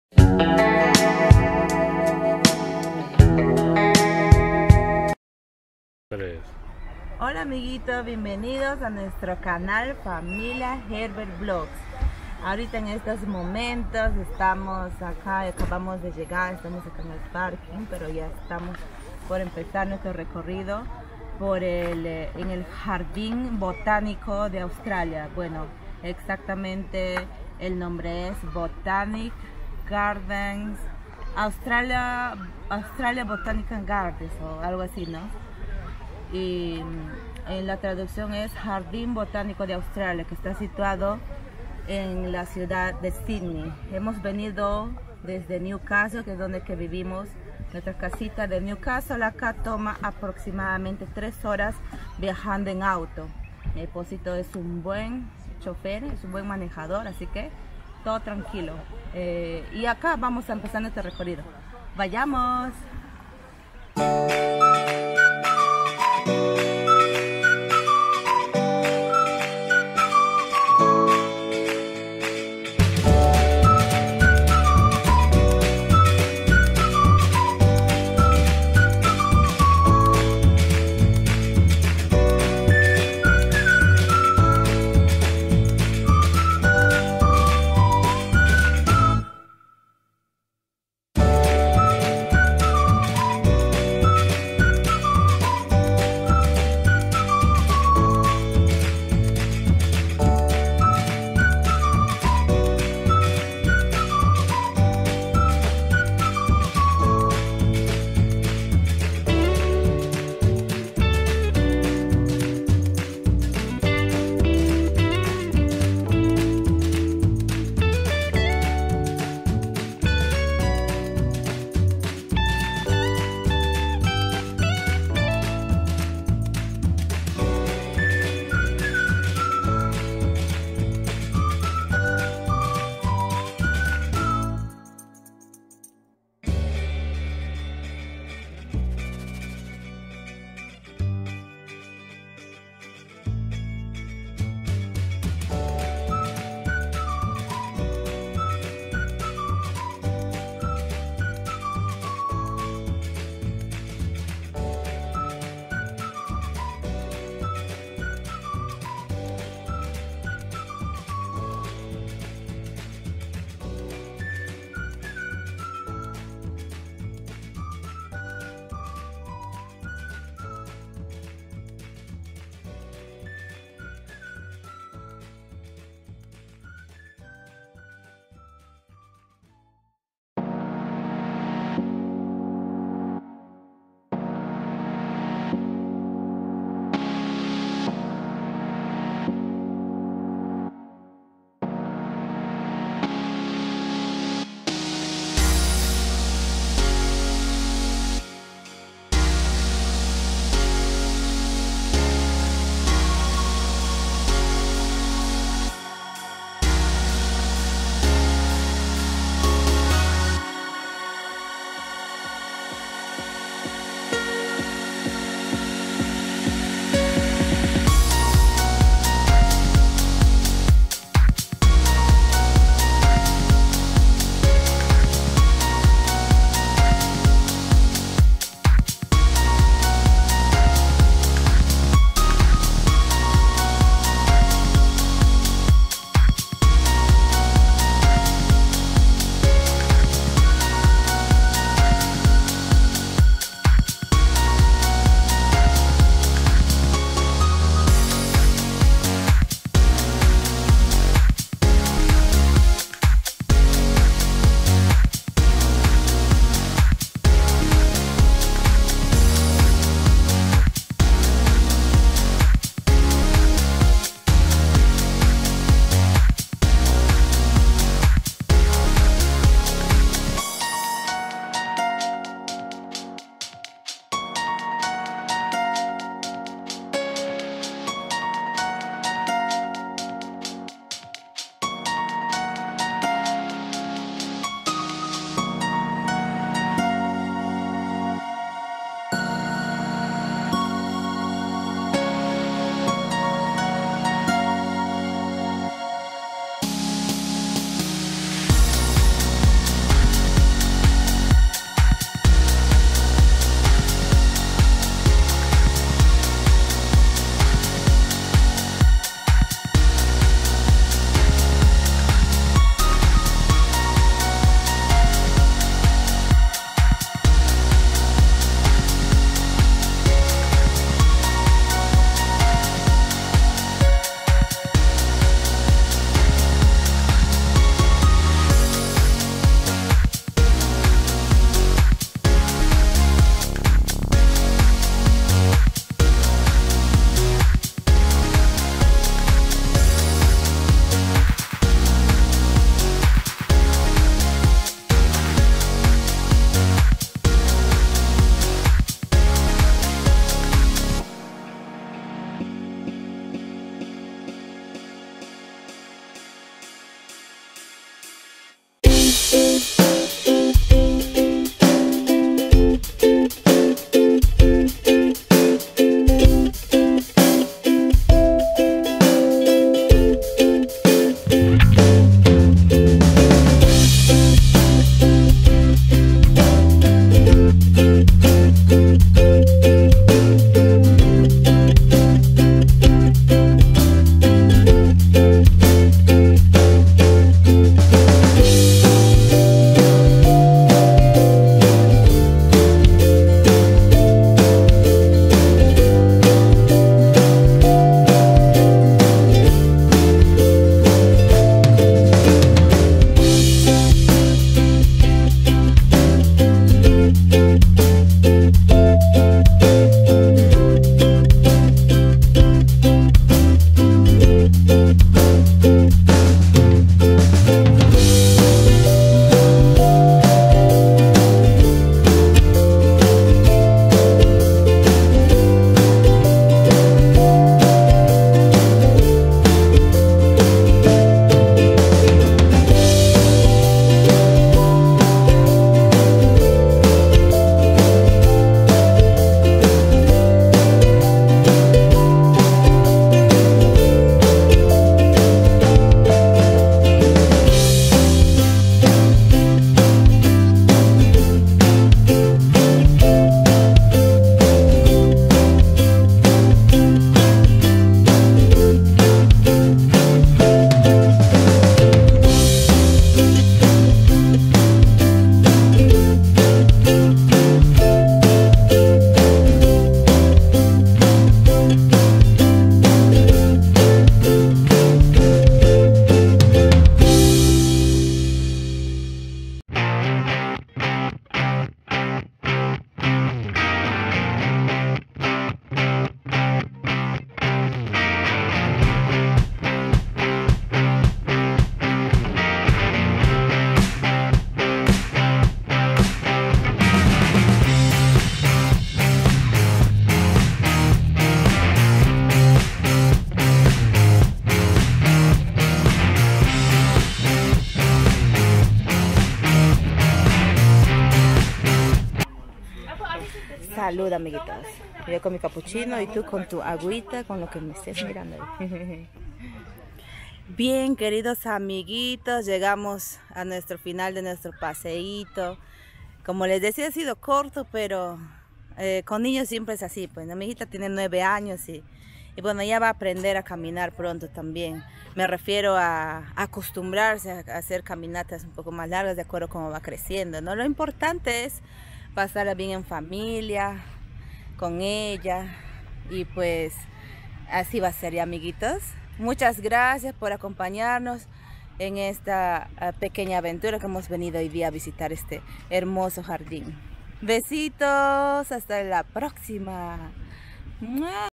JARDIN BOTANICO DE AUSTRALIA EN SYDNEY - 2022 🌸
Hola amiguitos (as), aquí les comparto mi video  paseando un rato en el jardín botánico de Australia , se llama exactamente " The Australian Botanic Garden Mount Annan " , que está situado en Sydney .Hemos pasado un bello momento allí , con mi esposo y mi hijita, espero disfruten ver este video , muchos abracitos y muchas bendiciones ❤💋❤

Hi my Dear friends, I share with you my video sightseeing in the Australian Botanic Garden , the right name is " The Australian Botanic Garden Mount Annan " , which is located in Sydney. We really spent a wonderful time there , with my husband and my daughter,  I really hope you enjoy this video , many hugs and blessings ❤💋❤